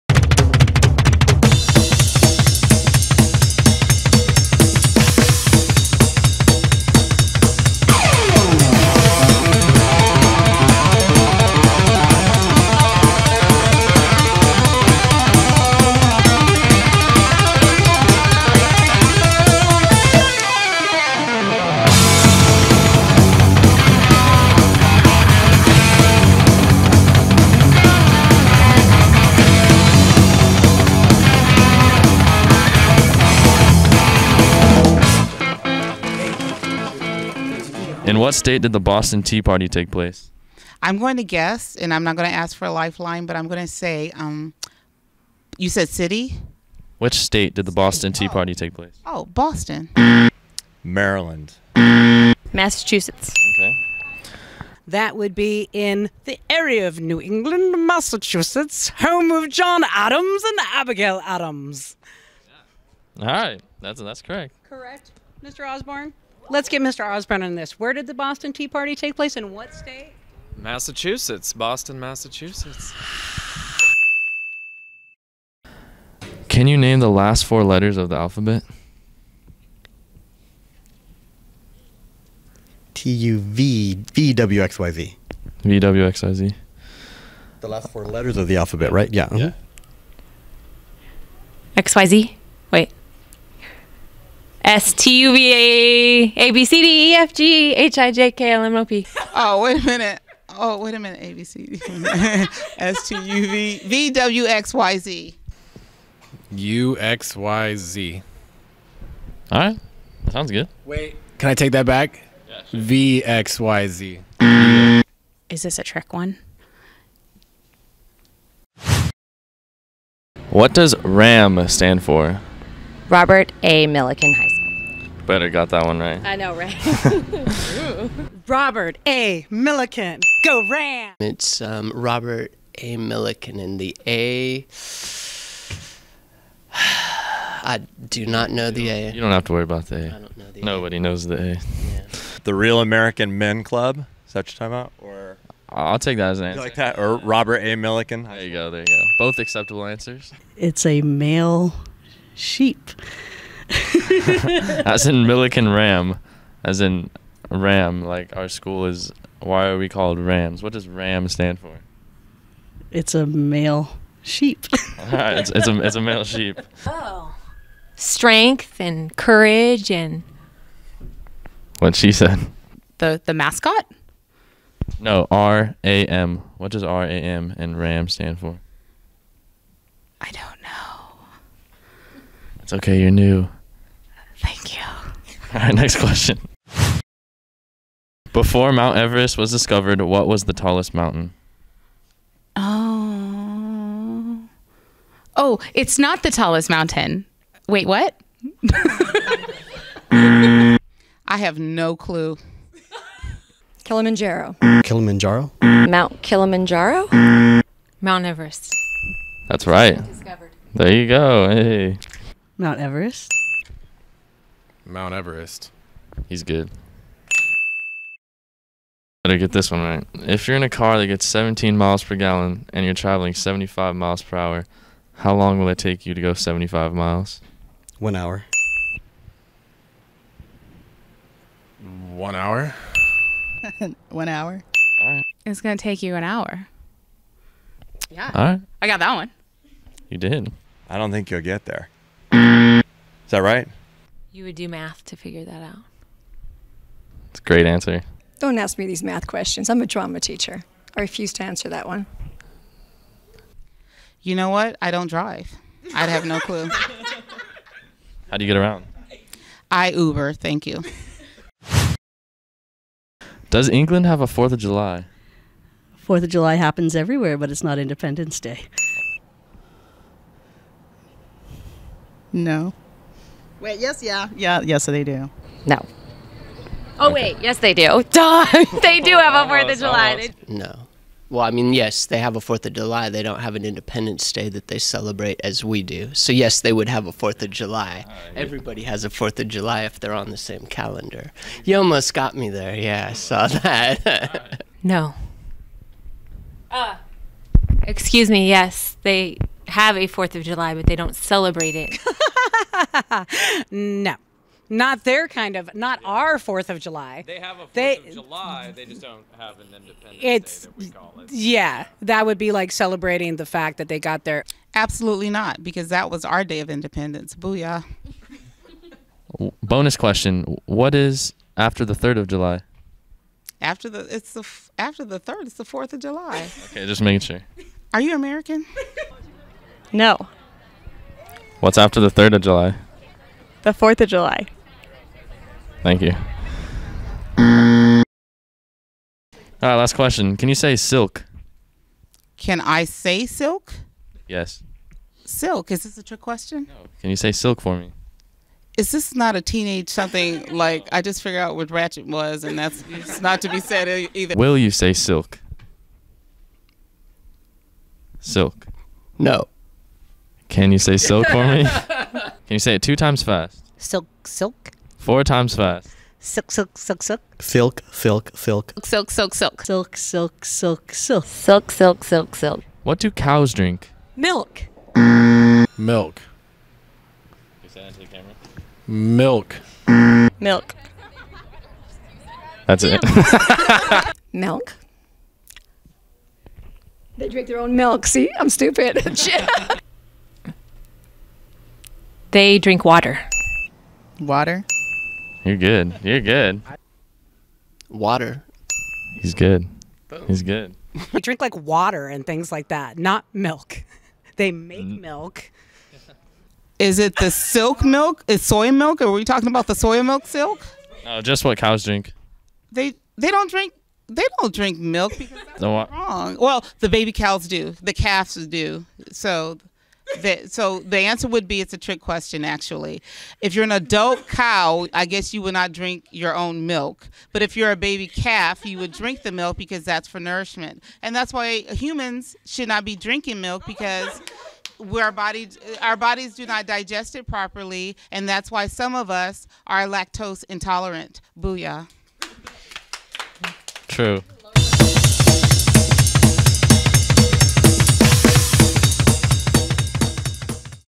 QUESTION with Clay | Teacher Edition
We ask Teachers questions they THINK they know and questions they SHOULD know.

Hosted By: Clayton Reynoso
Filmed & Edited By: German Lomeli - @TheGermanLomeli

ANSWERS:
1. SPAGHETTI
2. Massachusetts
3. WXYZ
4. Robert A. Millikan
5. Mt. Everest
6. 1 hour
7. Yes
8. Water